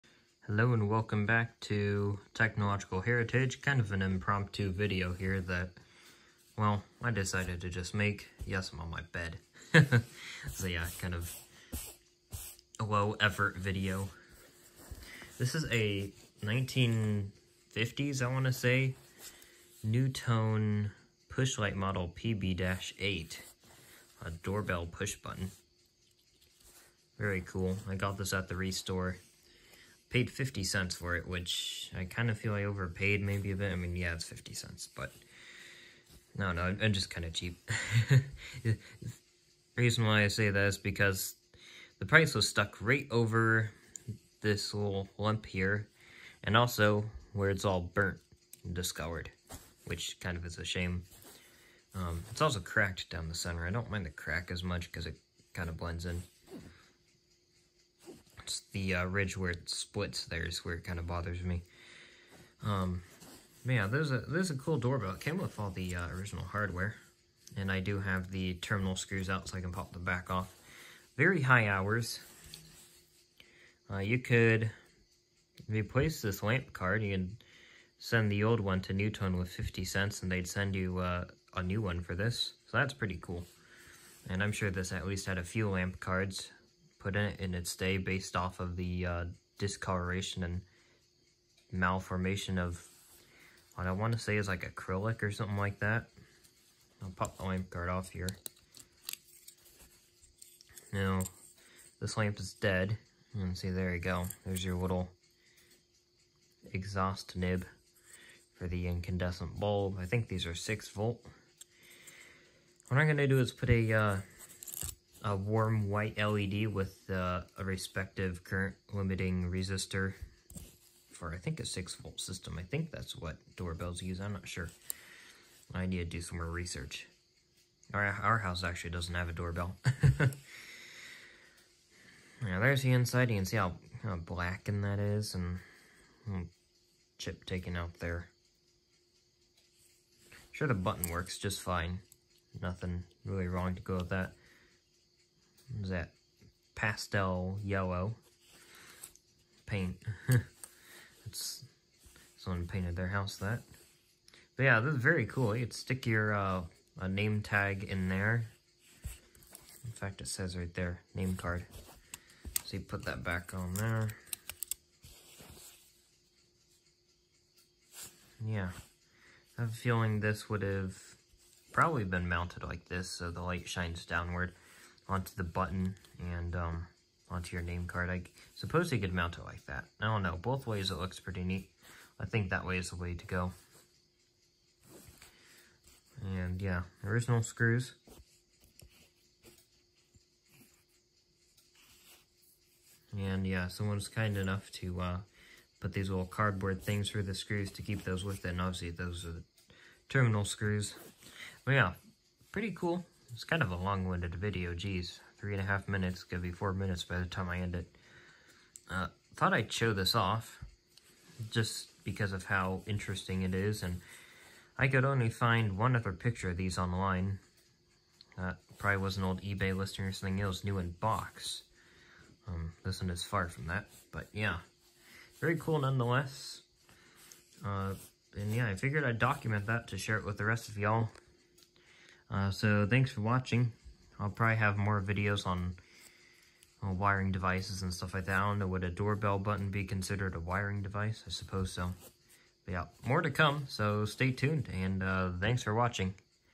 A neat old Nutone doorbell push button
Just one of those random showcase videos that I post on my channels, this time about a doorbell button lol￼. This is a high presume to be 1950s Nutone lighted doorbell button. Model PB-8. As someone who really likes the mid century, modern architectural style, I just had to grab it when I saw it.